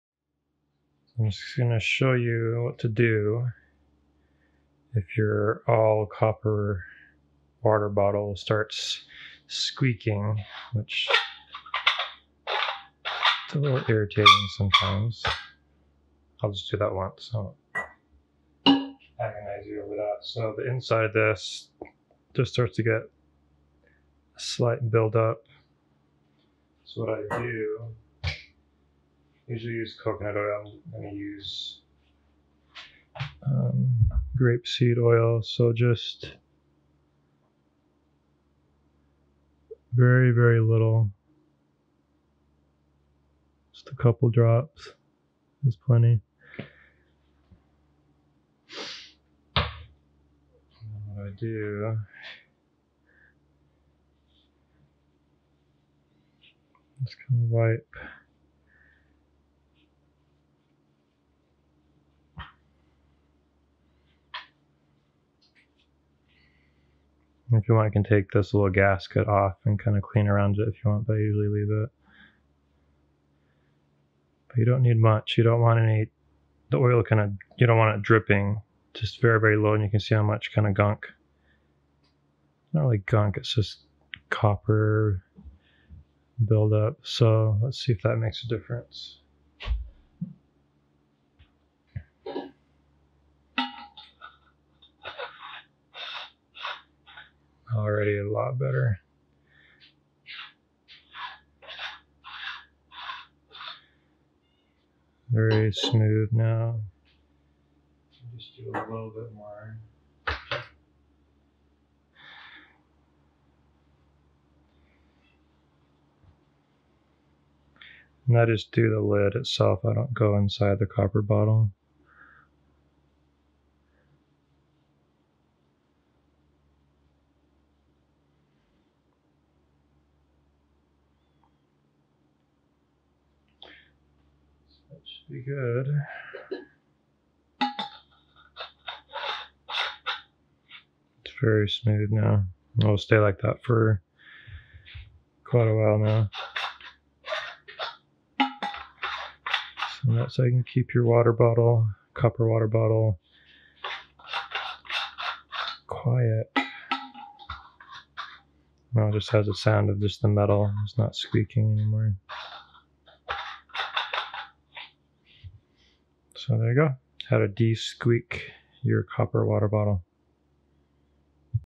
Fix Squeaky Copper Water Bottle
Quick fix to reduce the friction & Screeching sound on your copper water bottle.